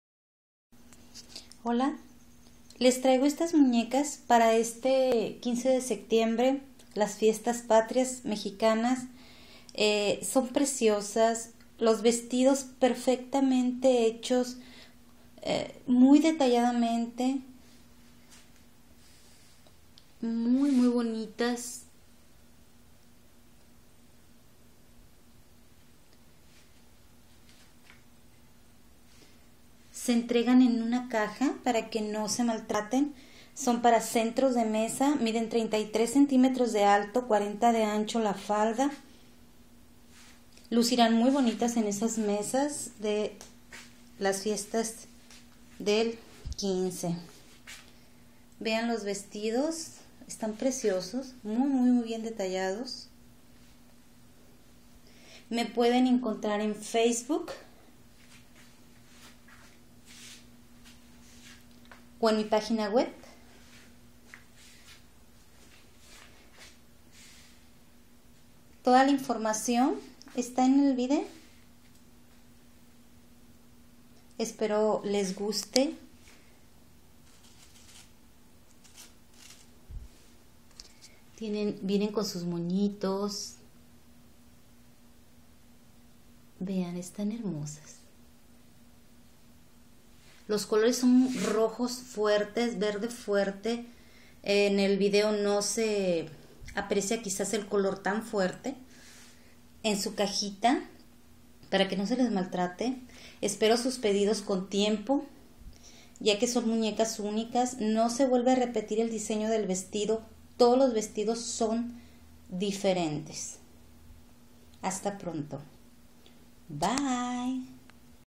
video 2 de centro de meza de muñequita mexicana
mide 33cm por 40 de ancho viene en sucaja
mi pagina web recuerdosdetalleshermosillo.mex.tl
facebok detalles y recuerdos hermosillo